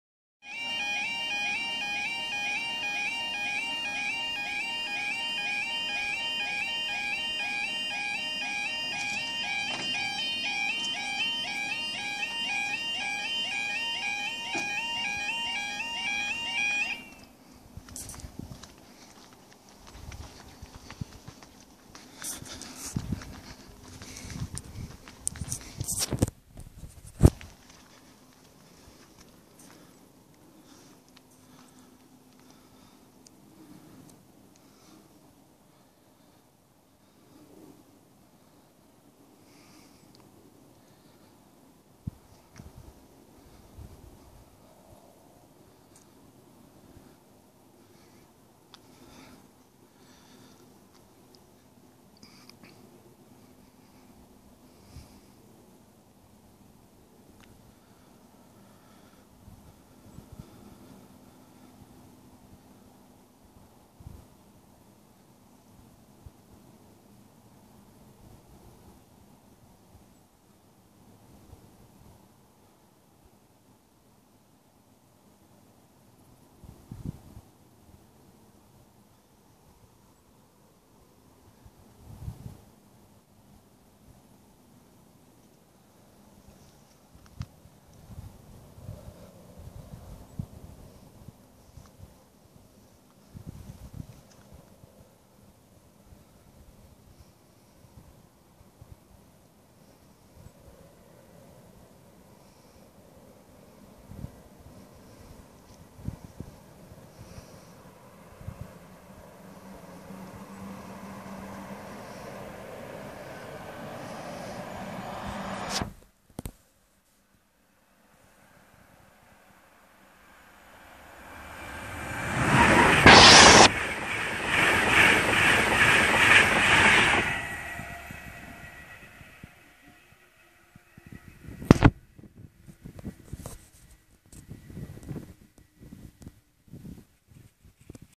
Connington lc in Cambridgeshire
Night shot of Connington level crossing which is said to be haunted, so much so that the locals won't go near at night